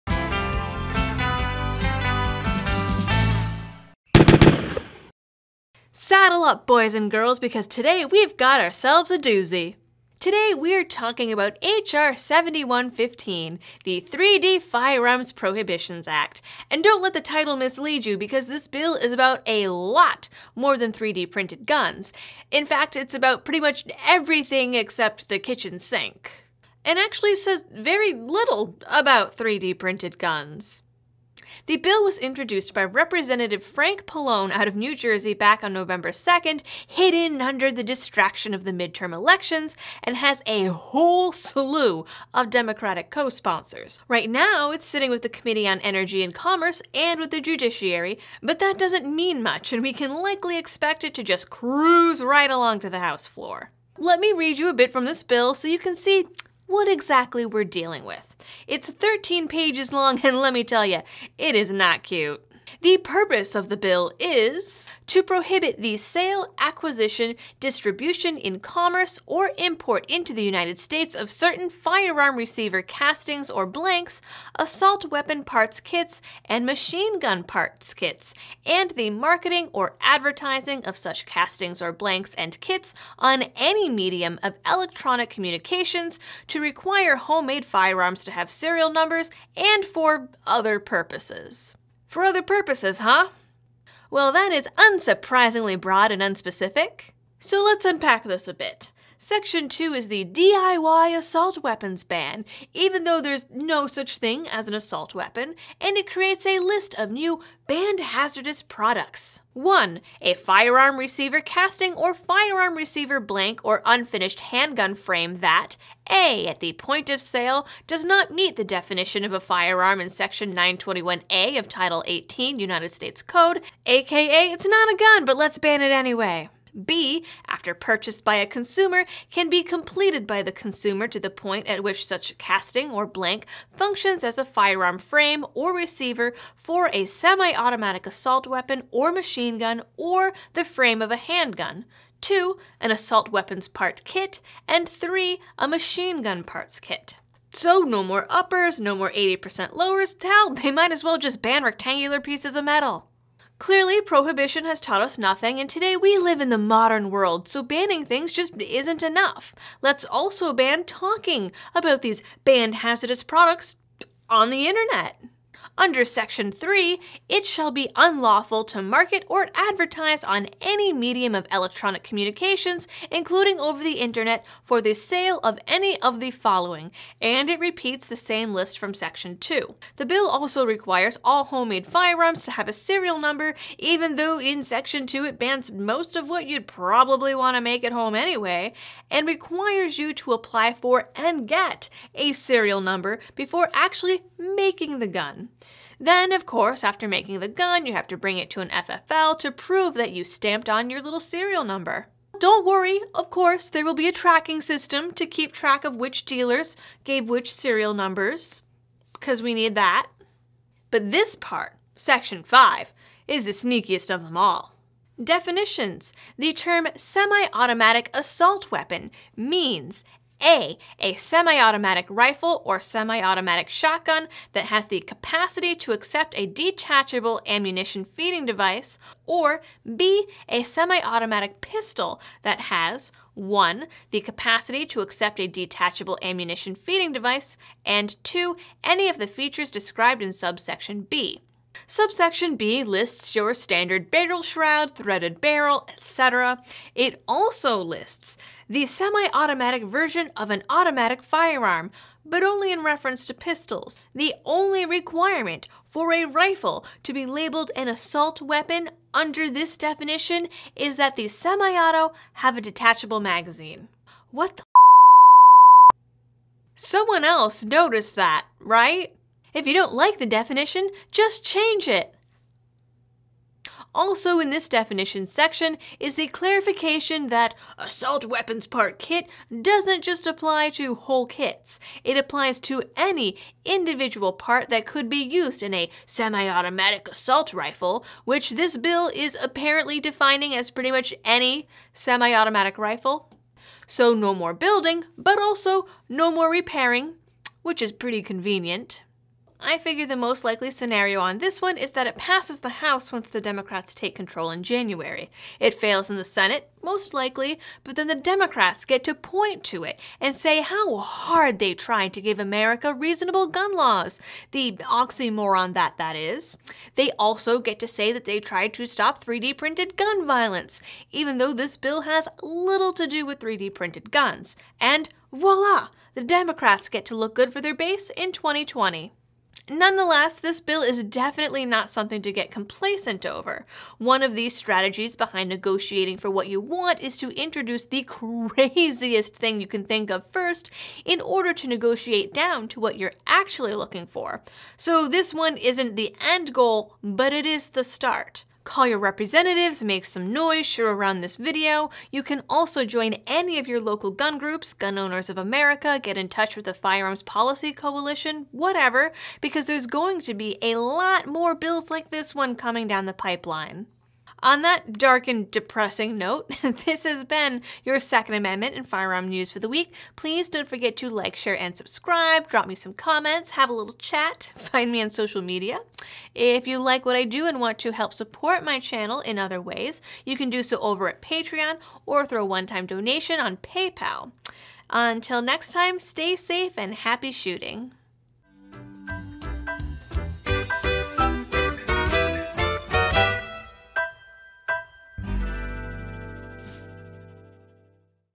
HR 7115: 3D Firearms & "DIY Assault Weapons" Ban
I discuss the newest dumpster fire of a gun control bill, HR 7115, the so-called "3D Firearms Prohibitions Act."

Bitcoin: 15qPy8TYAKTA2mZVhdh31Cc5bxxqmDYSho
Litecoin: ltc1qua8u3jz6qlax85f5ytjx0pepmdztd5npfgs34u

Liberty Doll
West Bridgewater, MA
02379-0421